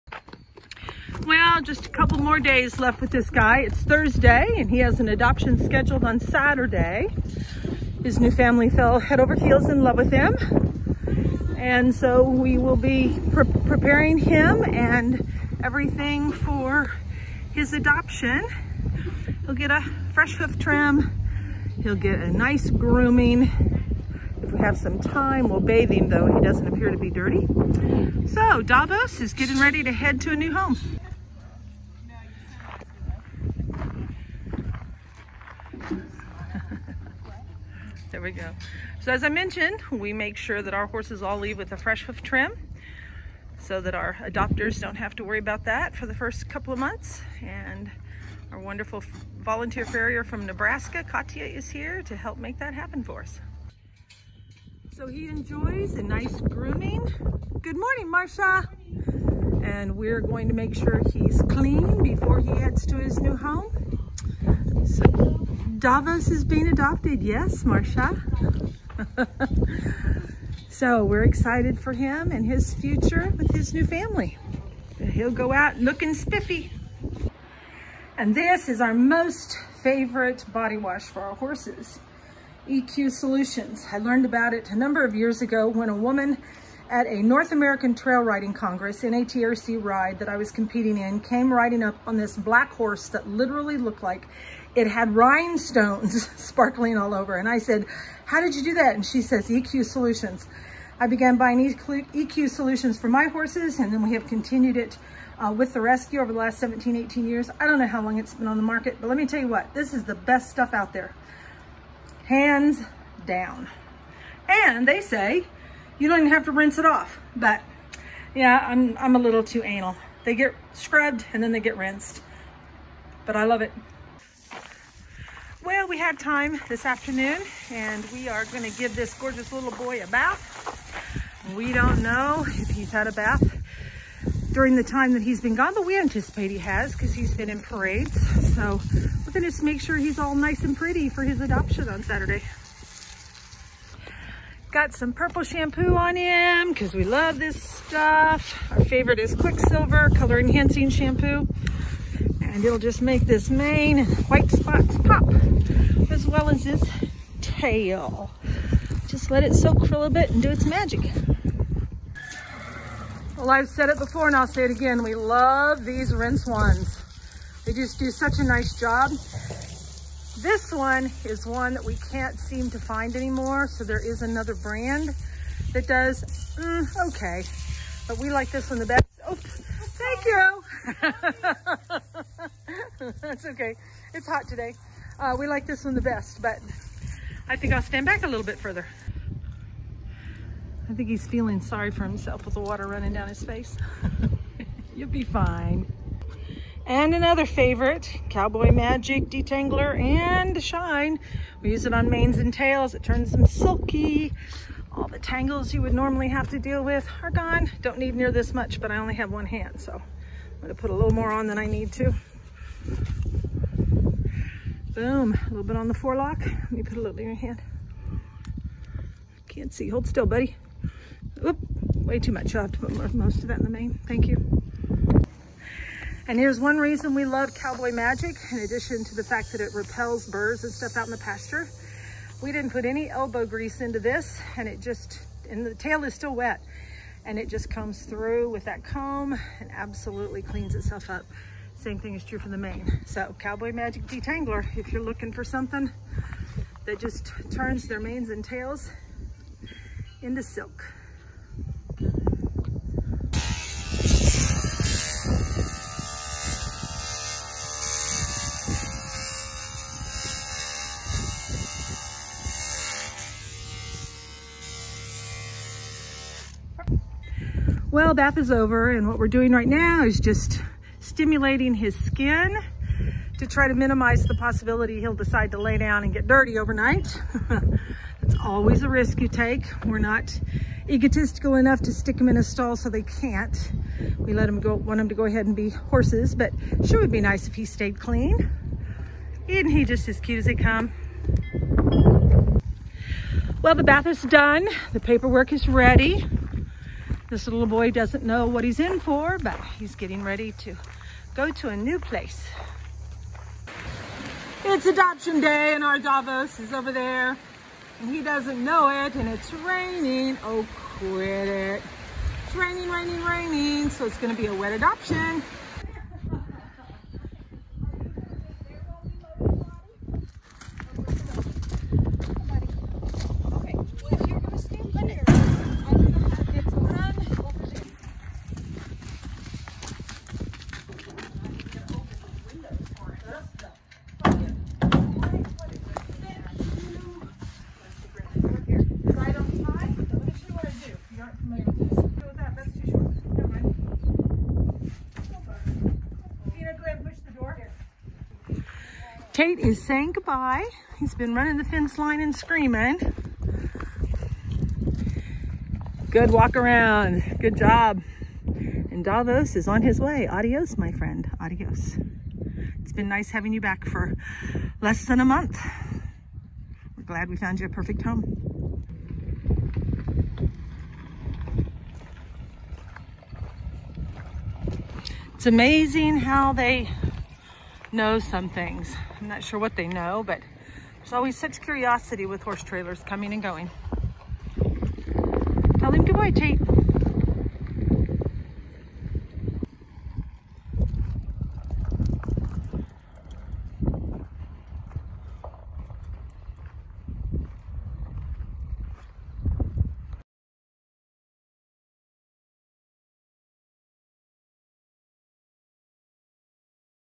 Adios Davos
Davos has only been back with us about a month and he has already found his new family.

He has been a consummate gentleman.

His quiet demeanor and cooperative spirit drew them to him.

Adios Davos.  Live a long and happy life.